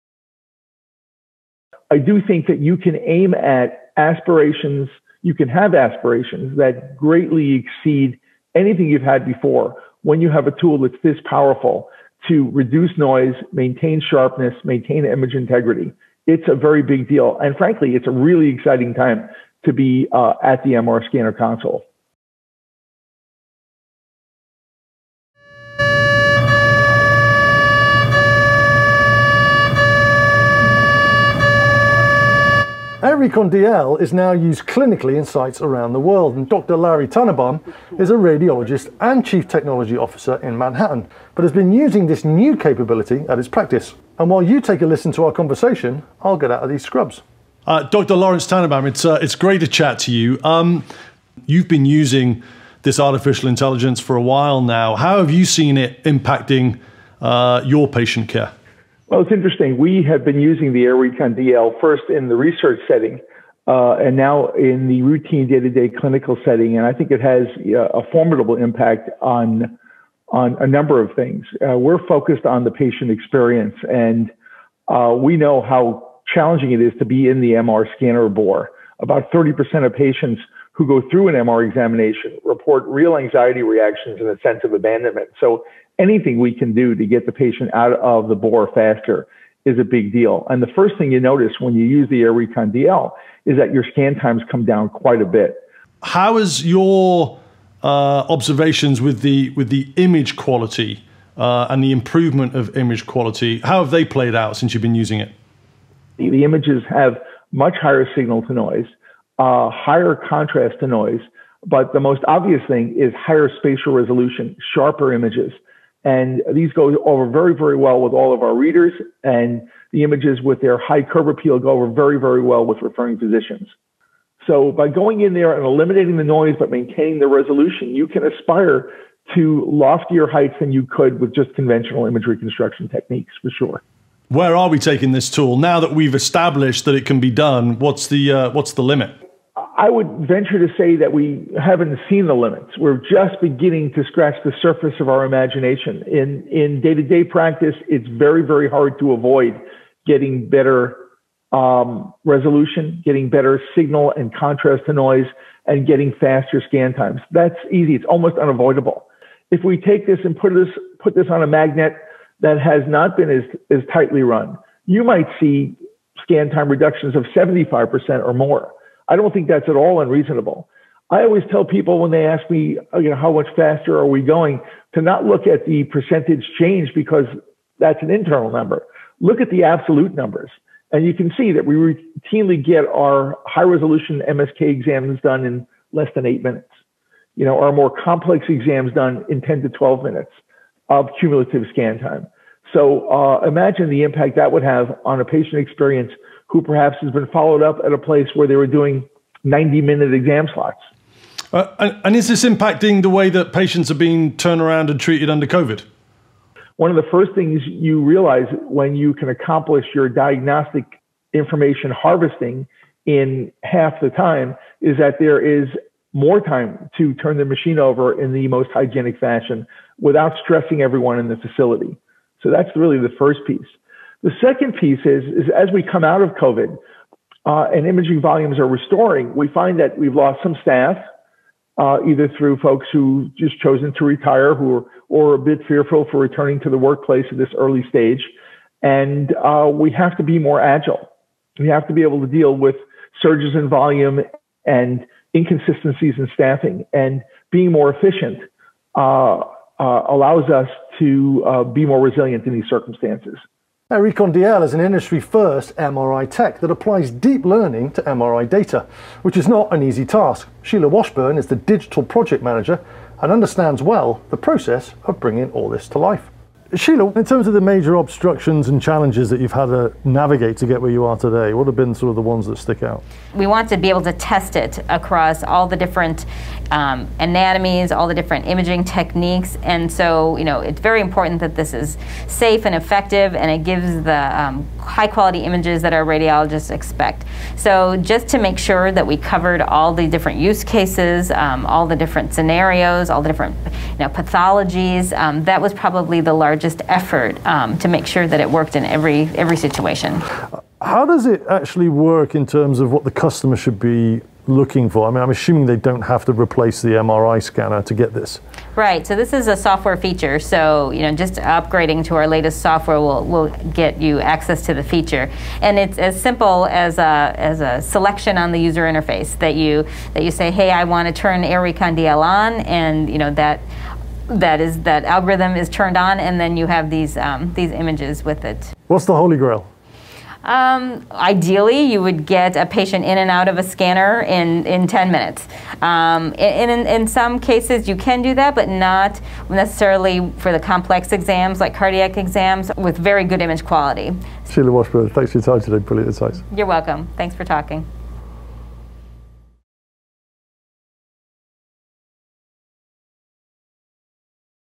Ep. 9 - Faster MRI scans and improved image quality | GE Healthcare
To finish this week’s MRI deep dive, Dr. Lawrence Tanenbaum, an early adopter of AIR Recon DL, explains the benefits for patient care in both research and routine clinical settings. With many scans that can now be done in 8-12 minutes, patients are already benefiting from this MRI innovation. Mikey Kay also speaks with product manager Sheila Washburn about the process of developing this industry-first tech and what the future may hold.